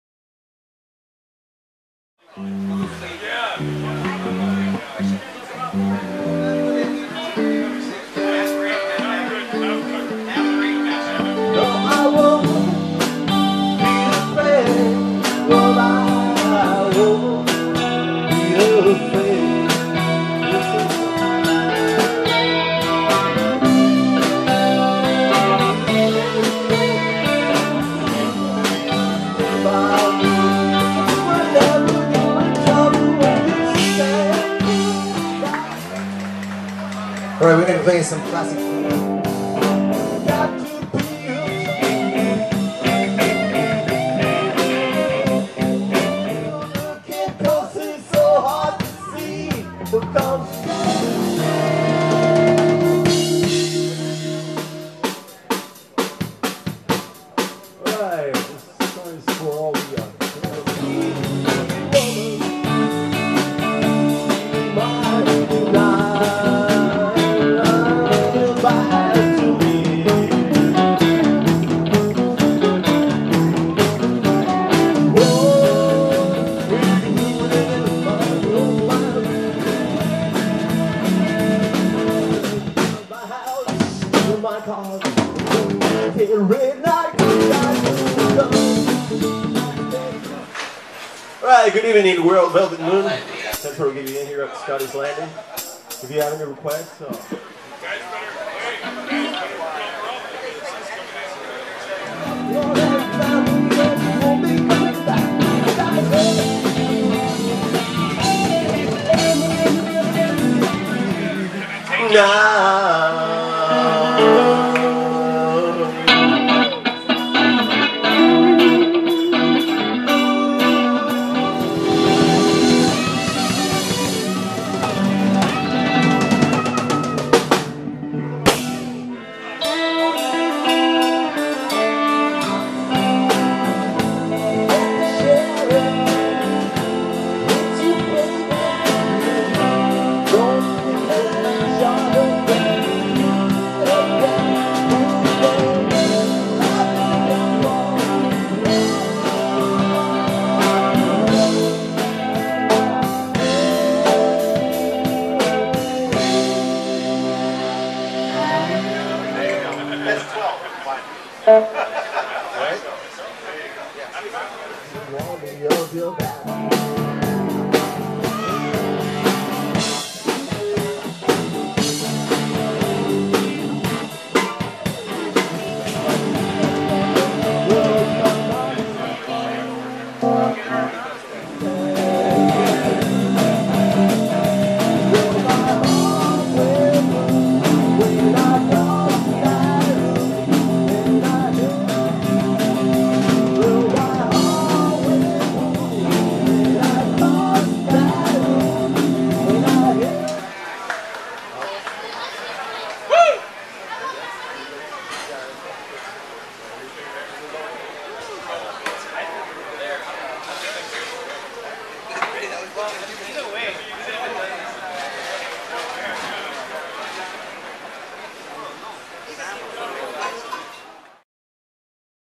Velvet Moon Live @ Scotty's Landing Sept. 4th 2010
The band Velvet Moon performing live at Scotty's Landing in Coconut Grove, South Florida. A summary of the first set.